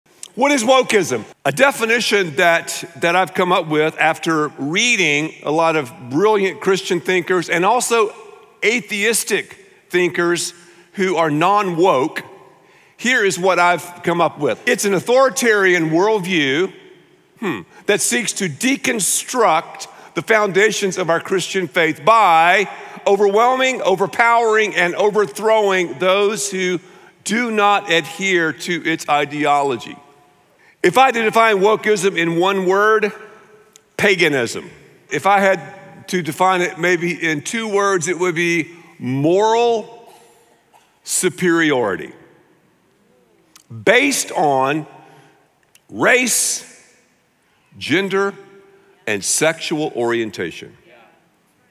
What is wokeism?
What is wokeism? | Ed Young

It’s an authoritarian worldview that seeks to deconstruct the foundations of our Christian faith by overwhelming, overpowering, and overthrowing those who do not adhere to its ideology.

Pastor Ed Young is the senior pastor of Fellowship Church based in the DFW Metroplex, he is also an author, and conference speaker noted for his creative communication style.